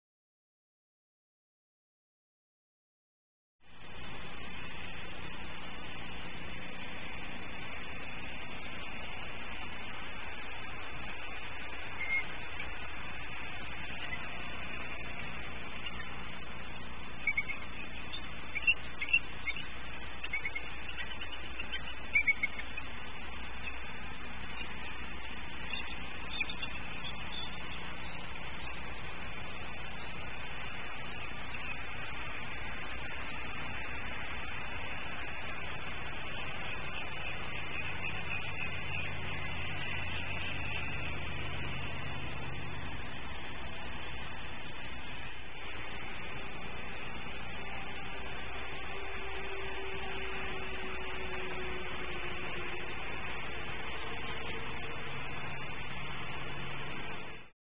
SW Florida Eagles_ Mating In The Nest Tree for Ozzie and Harriet 8:15am_9-30-13
Ozzie was perched in the attic above the nest...
He flew to the left side of the nest tree where Harriet was perched...
and a successful Mating occurred.